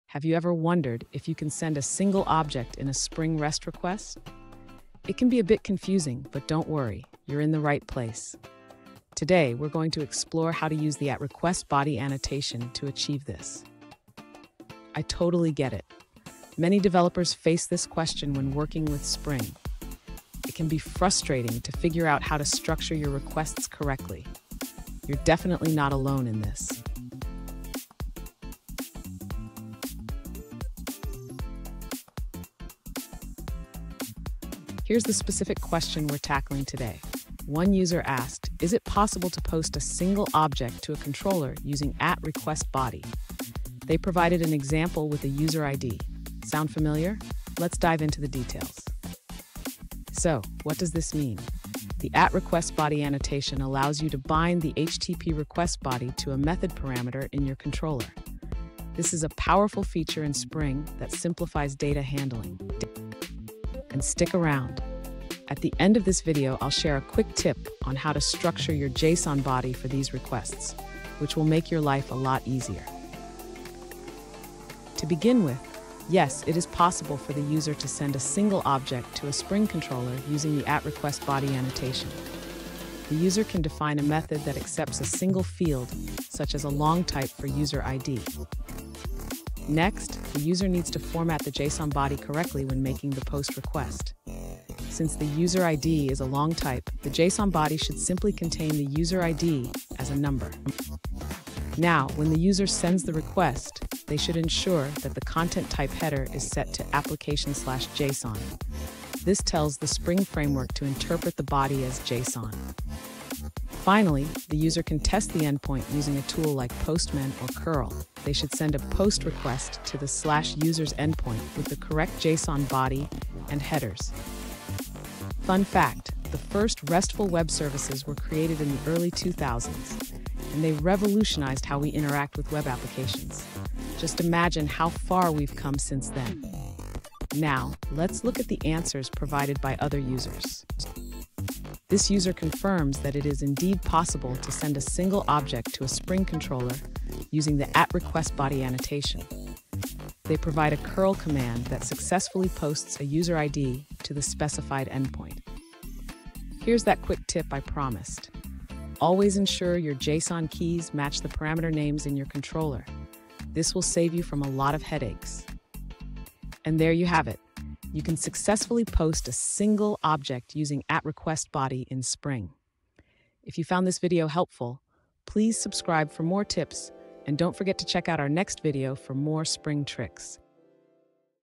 Handling Single Field Body in Spring REST Requests: Common Challenges & Solutions
In this video, we delve into the intricacies of handling single field bodies in Spring REST requests. As developers, we often encounter unique challenges when dealing with simplified data structures, and understanding how to effectively manage these scenarios is crucial for building robust applications. Join us as we explore common pitfalls and provide practical solutions to streamline your RESTful services.

Today's Topic: Handling Single Field Body in Spring REST Requests: Common Challenges & Solutions

Related to